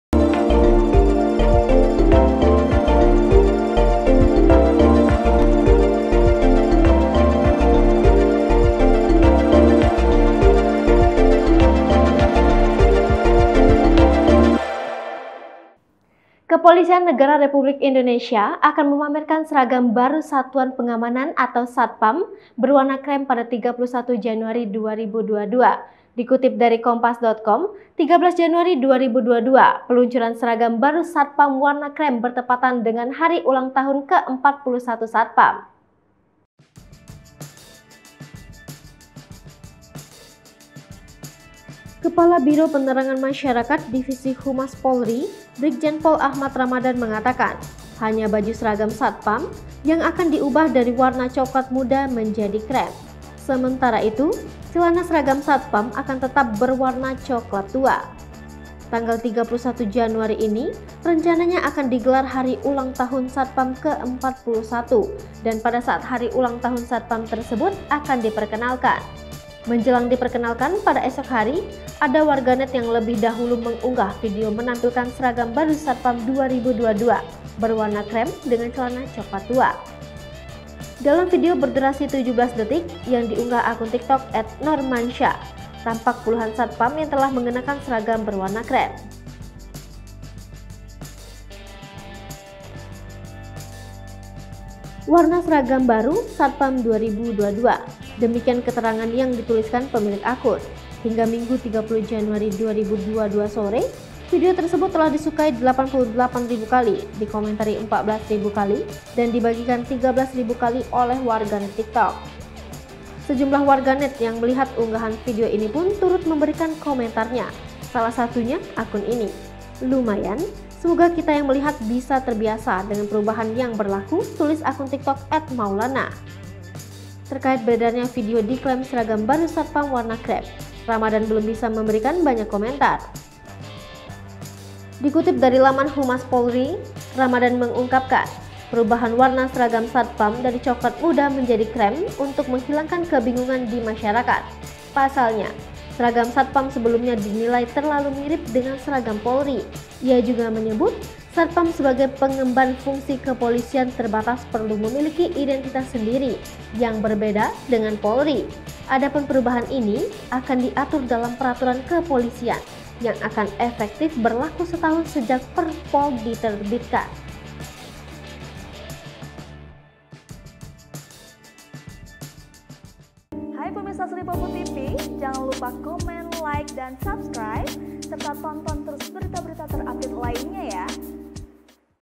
Penampakan Seragam Baru Satpam, Akan Dipamerkan Oleh Polri
Kepolisian Negara Republik Indonesia (Polri) akan memamerkan seragam baru Satuan Pengamanan (Satpam) berwarna krem pada 31 Januari 2022.

Dikutip dari Kompas.com, 13 Januari 2022, peluncuran seragam baru satpam warna krem bertepatan dengan hari ulang tahun (HUT) Ke-41 Satpam.

Kepala Biro Penerangan Masyarakat (Karo Penmas) Divisi Humas Polri, Brigjen Pol Ahmad Ramadhan mengatakan, hanya baju seragam satpam yang akan diubah dari warna cokelat muda menjadi krem.

Lihat informasi terkini bersama kami, di:

Palembang.tribunnews.com